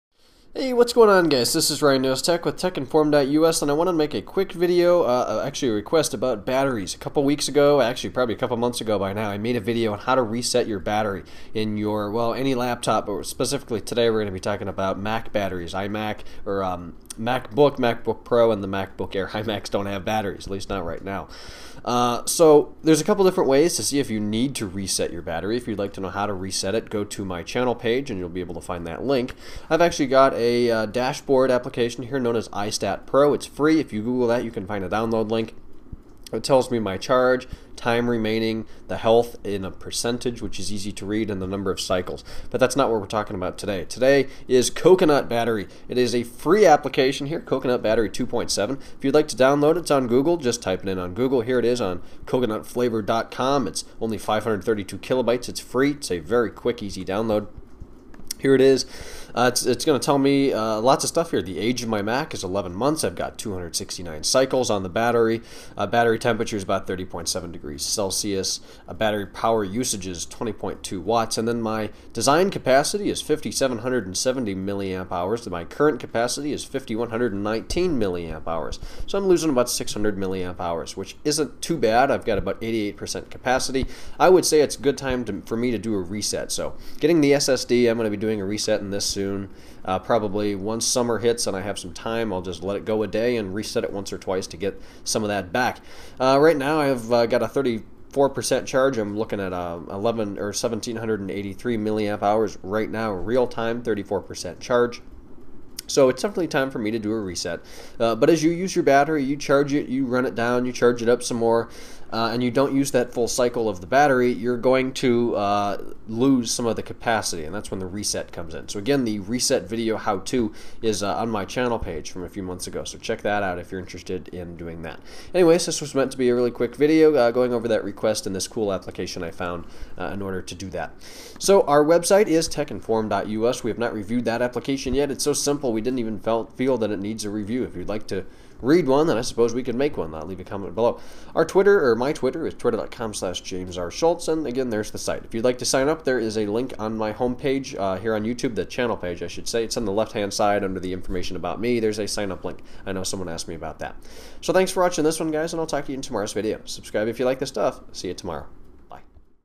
Coconut Battery Health Tool Review
A great application to check out your MacBook, MacBook Pros, or MacBook Airs battery life!